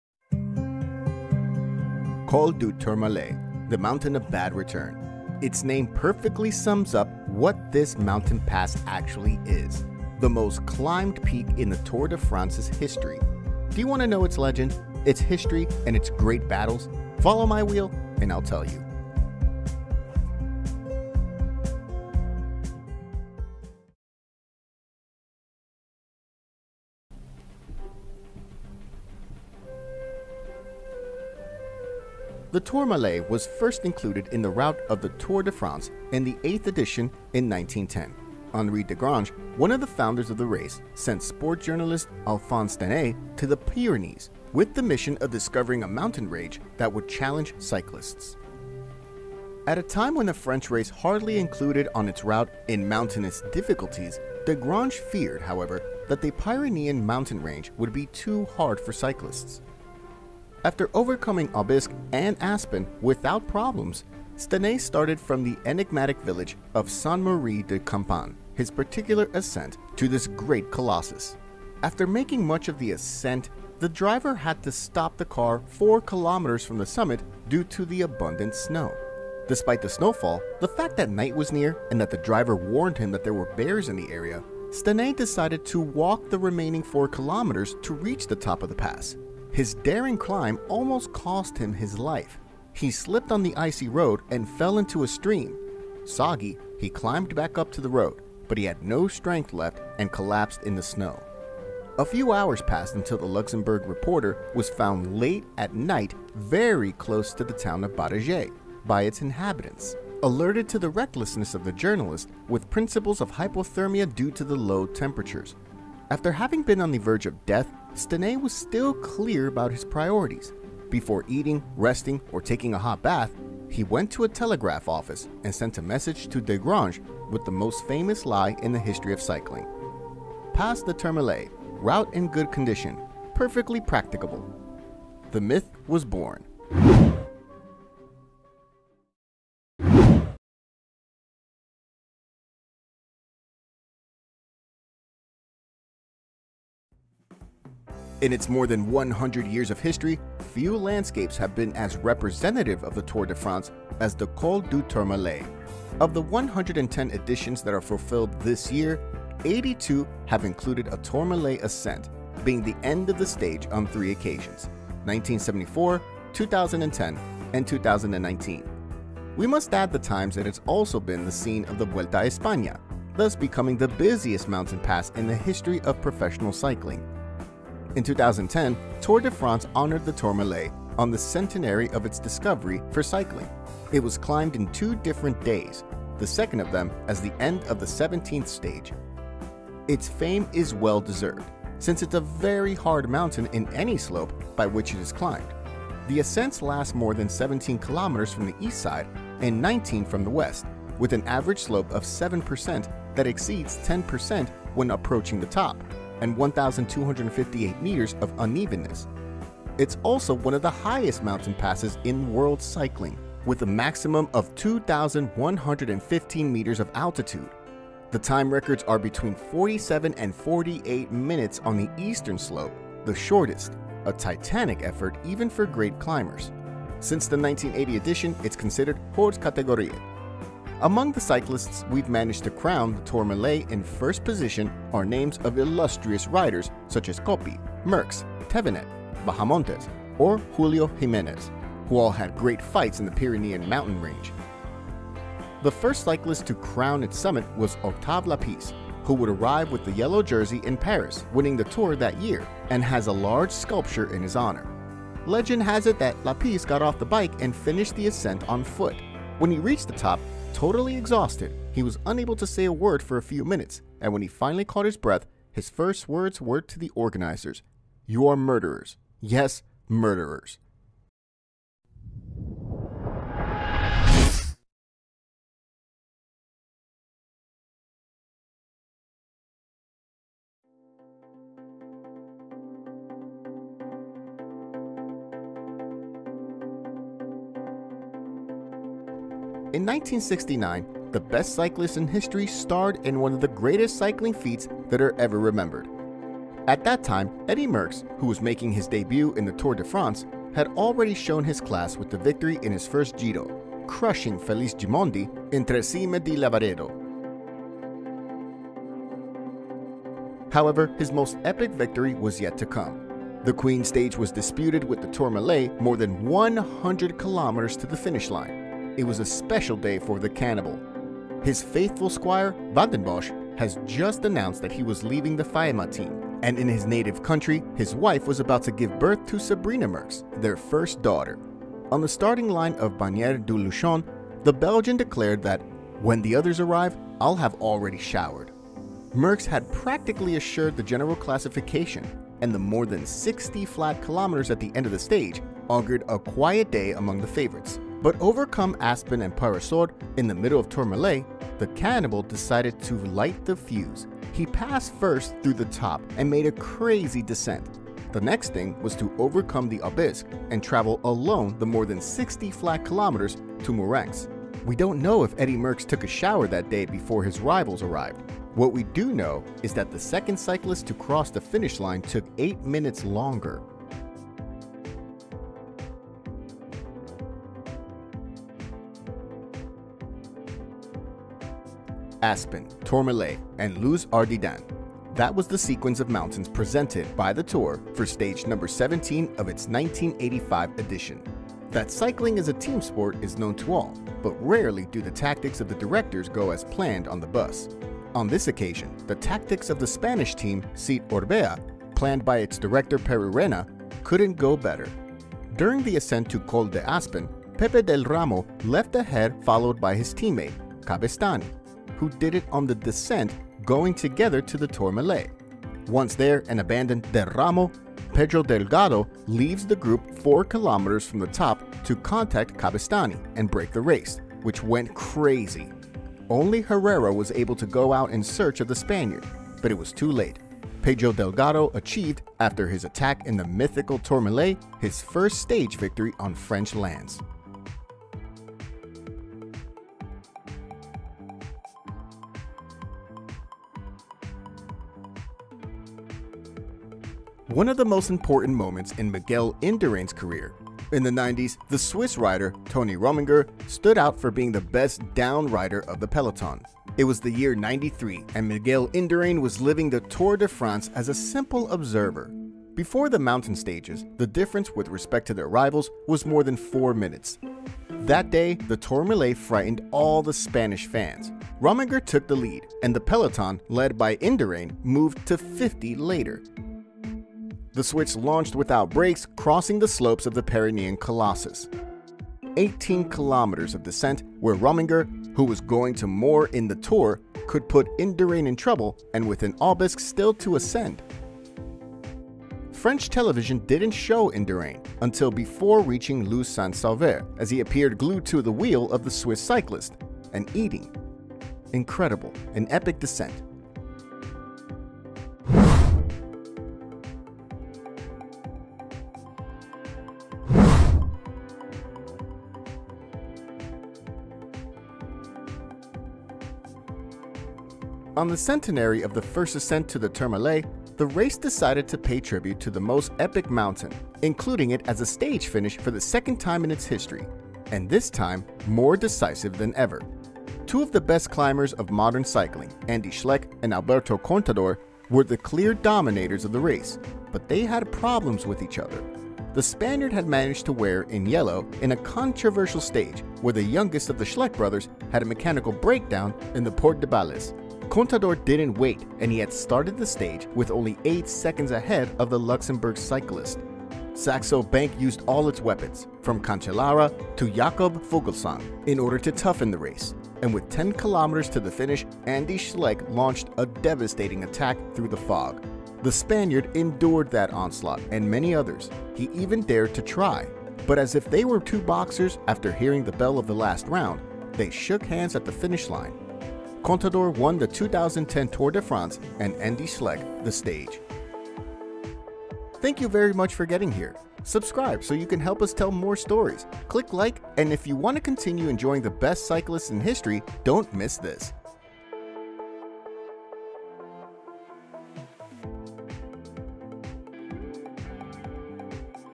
The big legend of the Tour de France - COL DU TOURMALET - History and great battles
Col du Tourmalet, the mountain of bad return. The most climbed pass in the history of the Tour de France. Get to know the legend and the best stories of this cycling myth.

In its more than one hundred years of history, few landscapes have been as representative of the Tour de France as the Col du Tourmalet. Of the 110 editions that are fulfilled this year, 82 have included a Tourmalet ascent, being the end of the stage on three occasions: 1974, 2010 and 2019. We must add the times it has also been the scene of the Vuelta a España, thus becoming the busiest mountain pass in the history of professional cycling.
In 2010, Tour de France honoured the Tourmalet on the centenary of its discovery for cycling. It was climbed in two differnts days, the second of them as the end of the 17th stage.
Its fame is well deserved, since it´s a very hard mountain in any slope by which it is climbed. The ascent lasts more than 17 kilometers from the east side and 19 from the west, with an average slope of 7% that exceeds 10% when approaching the top, and 1,258 meters of unevenness. It is also one of the highest mountain passes in world cycling, with a maximum of 2115 meters of altitude. The time records are between 47 and 48 minutes on the eastern slope, the shortest. A titanic effort even for great climbers. Since the 1980 edition it´s considered "hors categorie".